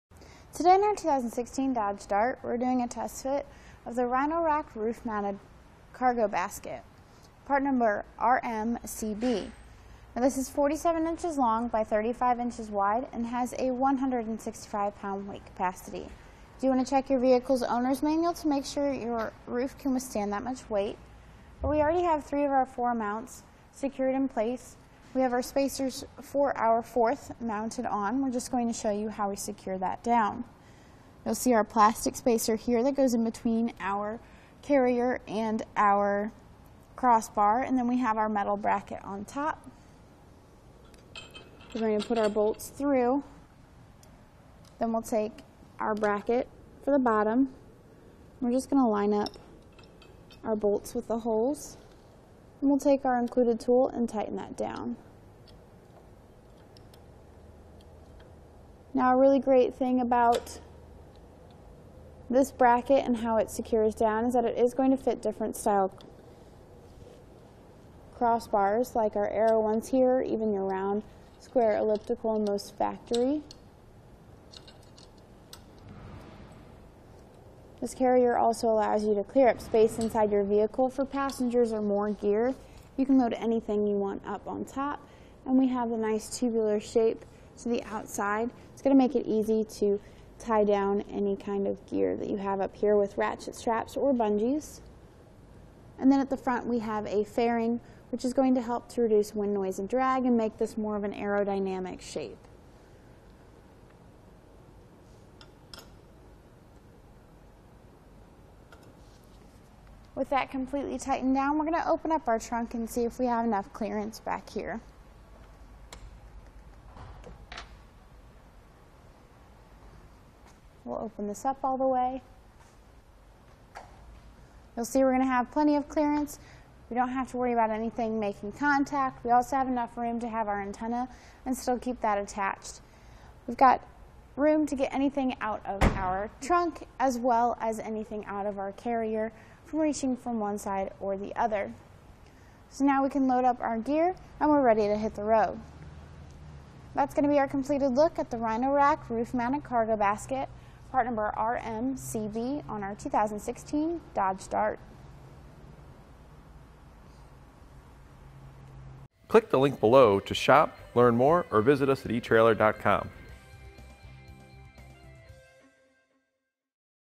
etrailer | Does the Rhino-Rack Roof Mounted Steel Cargo Basket Suit Your 2016 Dodge Dart
Today on our 2016 Dodge Dart we're doing a test fit of the Rhino-Rack roof-mounted cargo basket, part number RMCB. Now this is 47" long by 35" wide and has a 165 pound weight capacity. You do want to check your vehicle's owner's manual to make sure that your roof can withstand that much weight. We already have three of our four mounts secured in place. We have our spacers for our fourth mounted on. We're just going to show you how we secure that down.

You'll see our plastic spacer here that goes in between our carrier and our crossbar. Then we have our metal bracket on top. We're going to put our bolts through. Then we'll take our bracket through the bottom and we're just going to line up our bolts with the holes. We'll take our included tool and tighten that down. Now a really great thing about this bracket and how it secures down is that it is going to fit different style crossbars like our aero ones here or even your round, square, elliptical, and most factory.

This carrier also allow you to clear up space inside your vehicle for passengers or more gear. You can load anything you want up on top. We have the nice tubular shape to the outside. It's going to make it easy to tie down any kind of gear that you have up here with ratchet straps or bungees. Then at the front we have a fairing which is going to help to reduce wind noise and drag and make this more of an aerodynamic shape. With that completely tightened down we're going to open up our trunk and see if we have enough clearance back here.

We'll open this up all the way. You'll see we're going to have plenty of clear. We don't have to worry about anything making contact. We also have enough room to have our antenna and still keep that attached. We've got room to get anything out of our trunk as well as anything out of our carrier from reaching from one side or the other.

Now we can load up our gear and we're ready to hit the road. That's going to be our completed look at the Rhino-Rack roof-mounted cargo basket, part number RMCB, on our 2016 Dodge Dart.